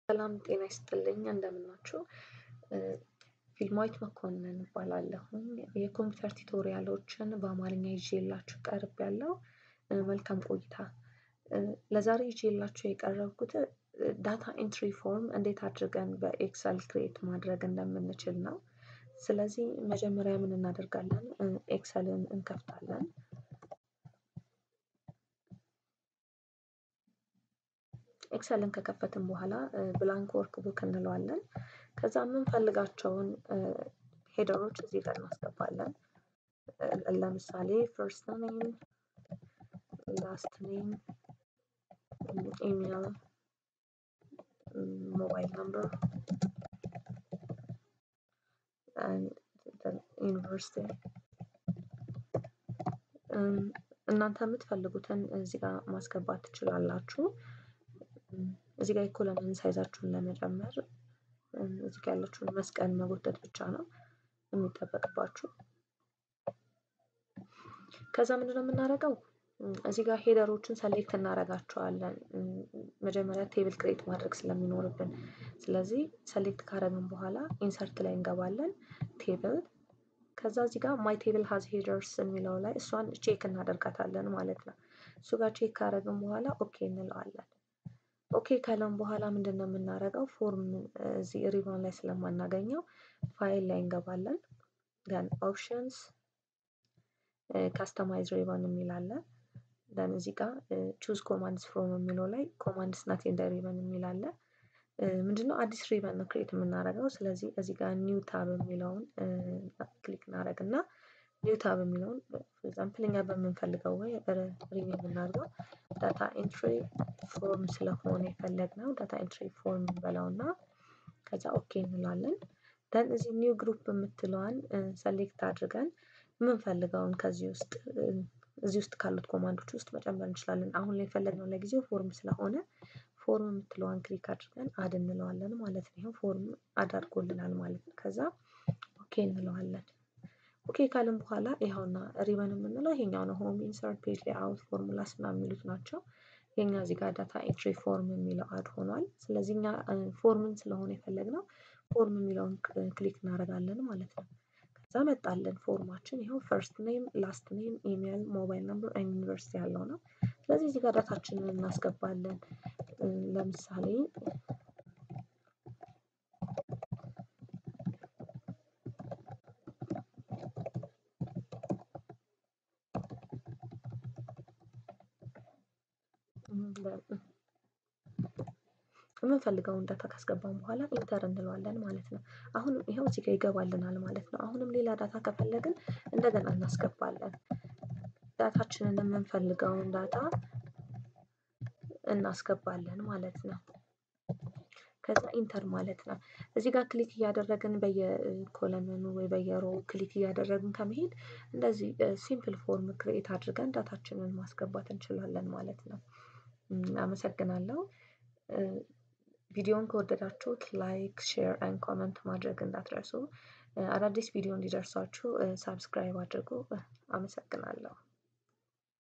How to create data entry form in Excel(Amharic)
In this video i am going to show you how to create data entry form in Excel.